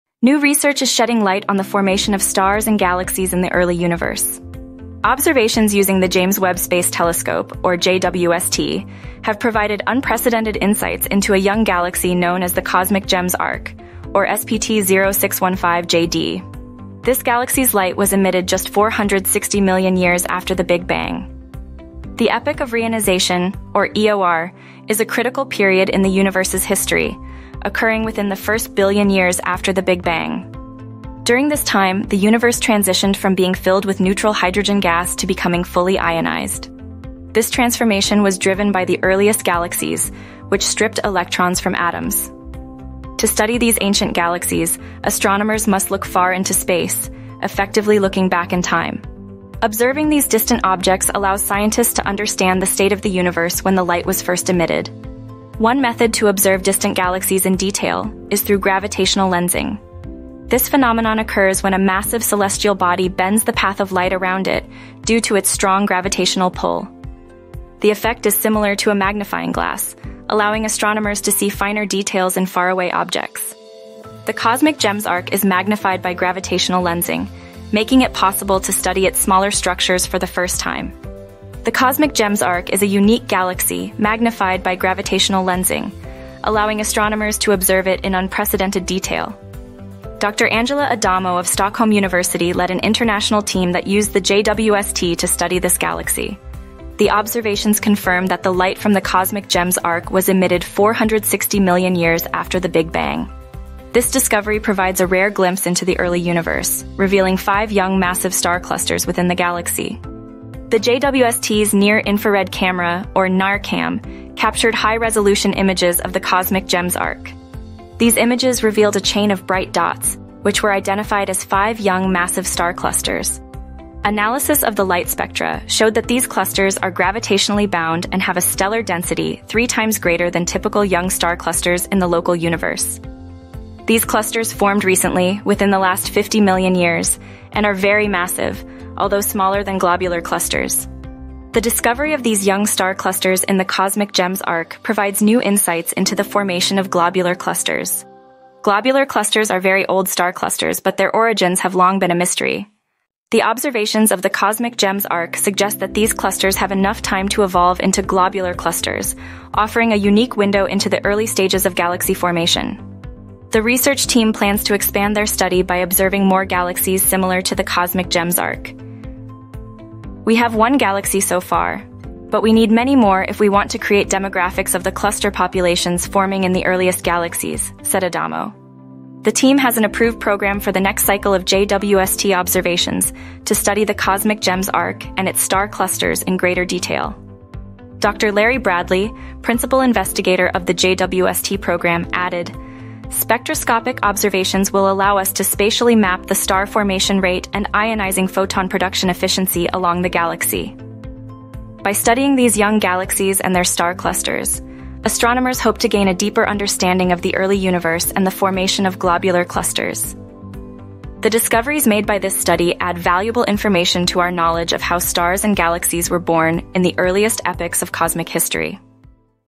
Unveiling Cosmic Origins with JWST
Astonishing findings from the James Webb Space Telescope reveal young star clusters in the Cosmic Gems arc, a galaxy formed just 460 million years after the Big Bang. 🌠 This galaxy, magnified by gravitational lensing, offers a rare glimpse into the early dynamics of galaxy formation. 🛰️

The discovery of these clusters might explain the precursors to globular clusters seen in mature galaxies today, pushing the boundaries of our cosmic knowledge. 🤯

This pivotal observation using JWST not only enriches our understanding of star and galaxy evolution but also marks a significant milestone in astrophysics. 🌌🔭

Source : Angela Adamo, Larry D. Bradley, Eros Vanzella, Adélaïde Claeyssens, Brian Welch, Jose M. Diego, Guillaume Mahler, Masamune Oguri, Keren Sharon, Abdurro’uf, Tiger Yu-Yang Hsiao, Xinfeng Xu, Matteo Messa, Augusto E. Lassen, Erik Zackrisson, Gabriel Brammer, Dan Coe, Vasily Kokorev, Massimo Ricotti, Adi Zitrin, Seiji Fujimoto, Akio K. Inoue, Tom Resseguier, Jane R. Rigby, Yolanda Jiménez-Teja, Rogier A. Windhorst, Takuya Hashimoto, Yoichi Tamura. Bound star clusters observed in a lensed galaxy 460 Myr after the Big Bang. Nature, 2024; DOI: 10.1038/s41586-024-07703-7